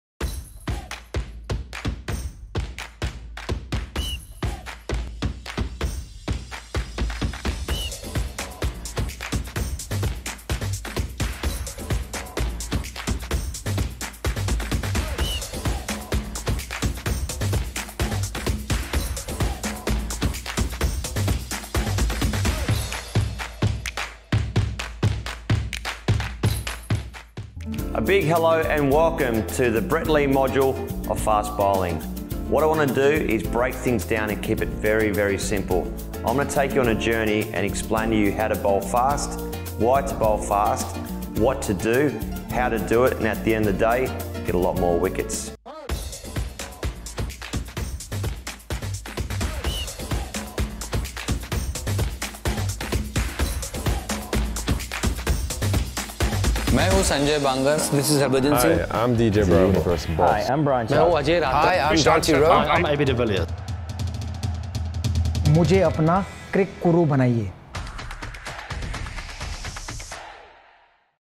Meet Our Cricurus- Brett Lee
Join us on this journey with Brett Lee, as the speedster takes us into the world of fast-bowling! To learn how to bowl fast and pick more wickets,